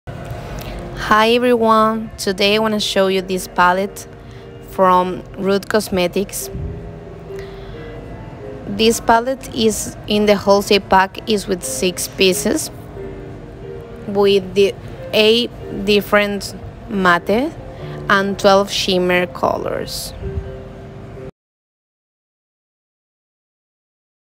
Wholesale Rude Cosmetics The Roaring 20'S Eyeshadow Palette - Neons l Wholesale Makeup
Wholesale Rude cosmetics The Roaring 20's Eyeshadow Palette - Neons comes in a 6 piece pack.
Neon Shades for the Bold and rebellious.
20 Pressed pigment and Eyeshadows.
8 Matte - 12 shimmer.
Cruelty Free.
0.63 oz (x12)
0.42 oz (x8)

3390 NW 168TH ST.
MIAMI GARDENS,